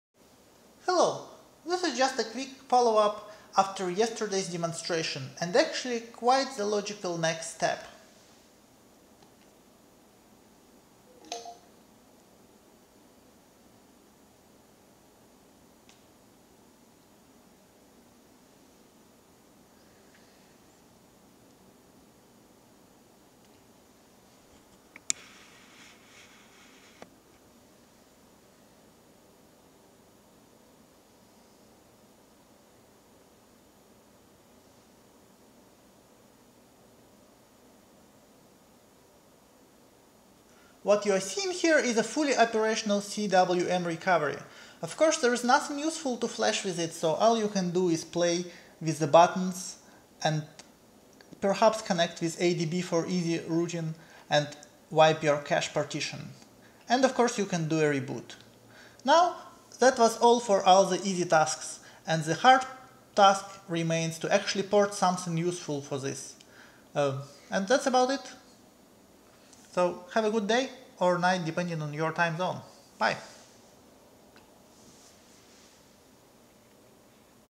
Nook HD+ CWM recovery
This demonstration concludes all the easy tasks for the Nook HD+ tablet.

CWM is fully operational and is now waiting until there's something substantial to flash with it.

Also I really hope B&N will get the hint and will stop the locked bootloader stuff, as I really hate to think I am actually condoning this awful behavior by giving people more reasons of buying these devices.

Buy Nexus devices, support openness!